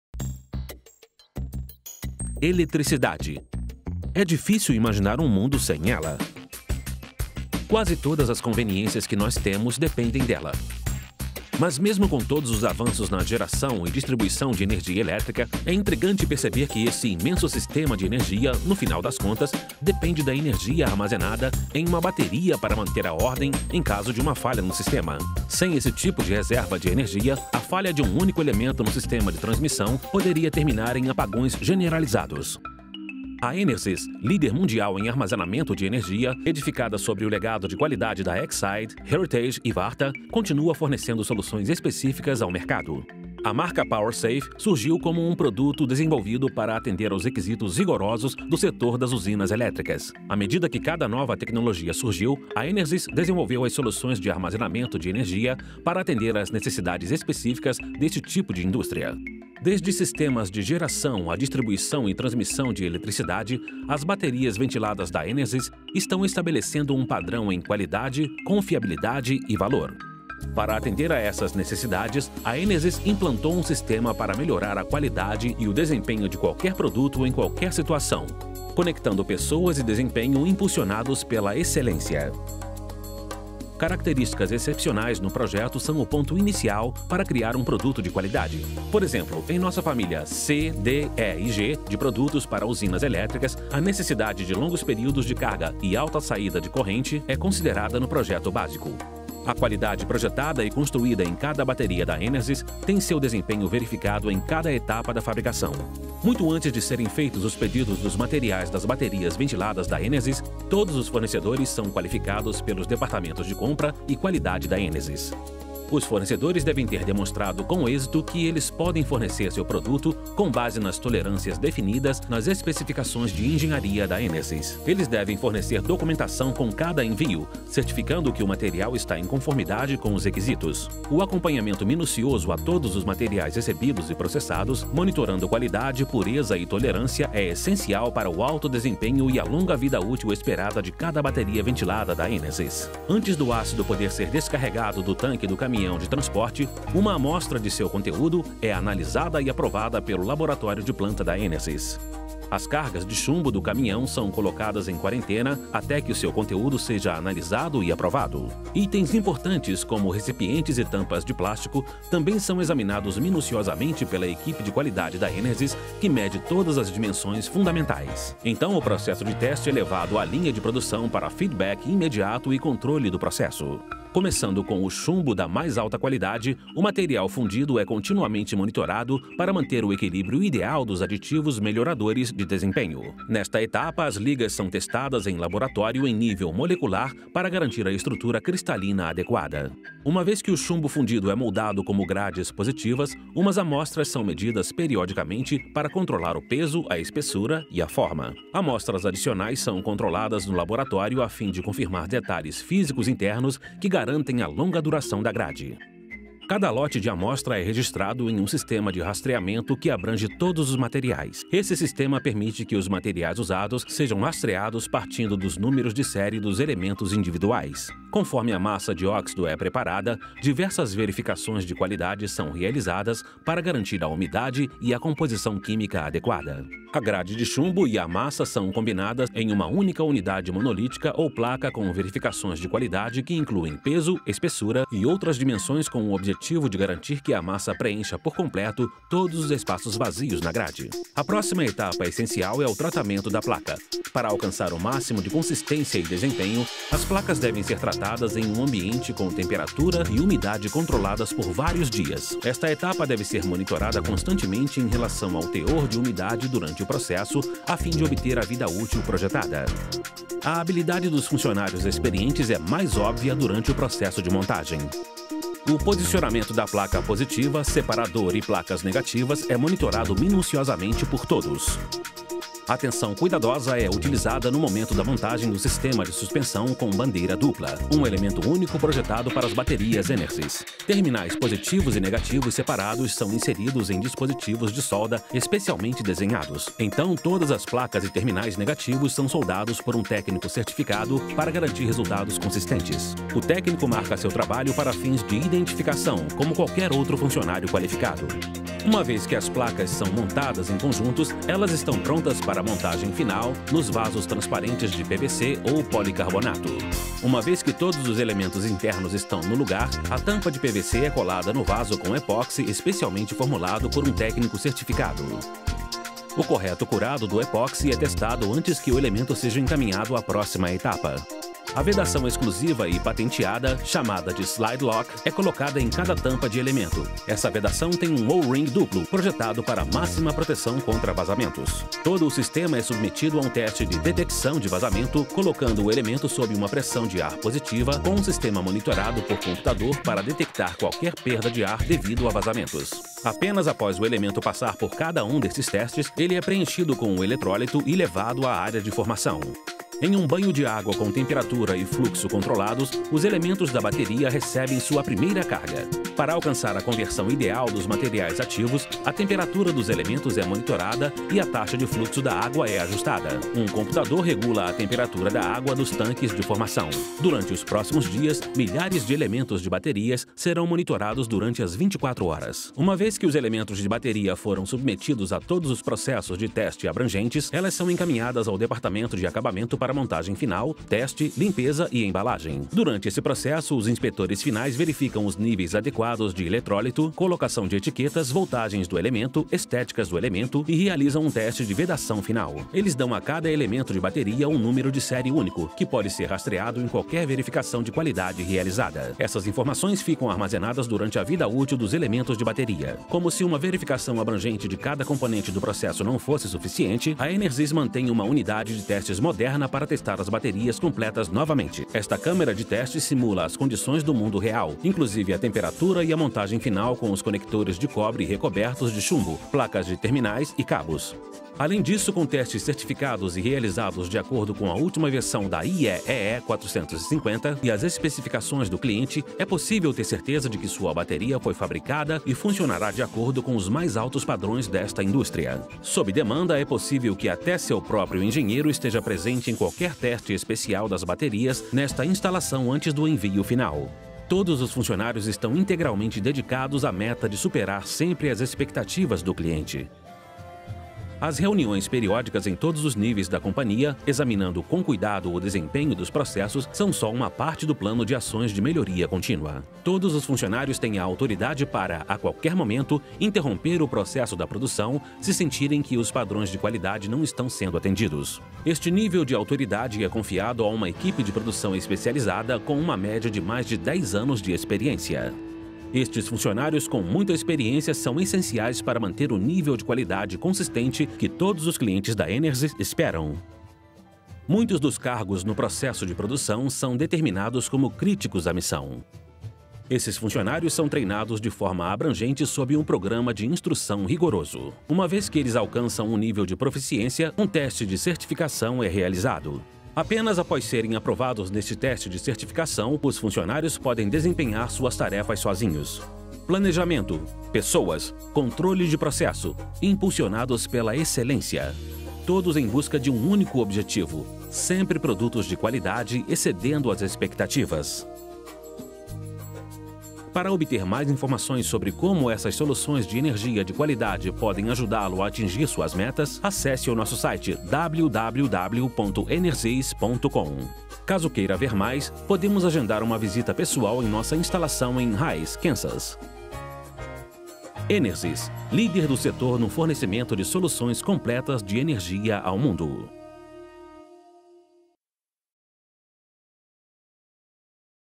Hays Plant Tour - Utility - Portuguese
Learn more about our energy storage solutions for Utility applications from this tour of our manufacturing plant in Hays, Kansas.  (Narration in Portuguese)